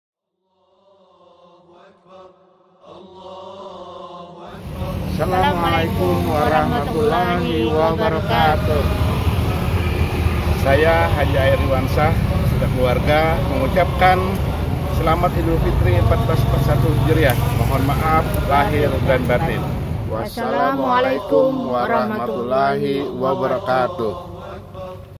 SuratKabarDigital - H AR Iwansyah - Idul Fitri 1441 Hijriah - Banjarbaru
Ucapan Lebaran Idul Fitri 1441 H oleh Ketua DPD Partai Golkar Kota Banjarbaru H AR Iwansyah